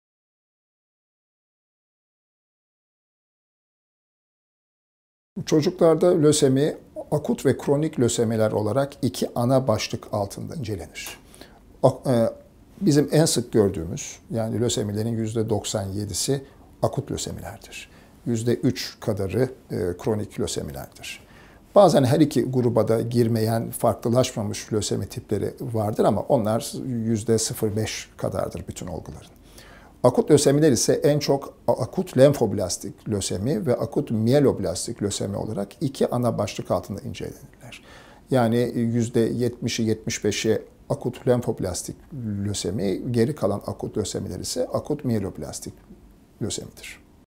Çocuklarda görülen löseminin farklı tipleri var mıdır?
Acıbadem Altunizade Hastanesi Çocuk Hastalıkları ve Hematoloji Uzmanı Prof. Dr. Cengiz Canpolat, Çocuk Hastalıkları üzerine soruları yanıtlıyor.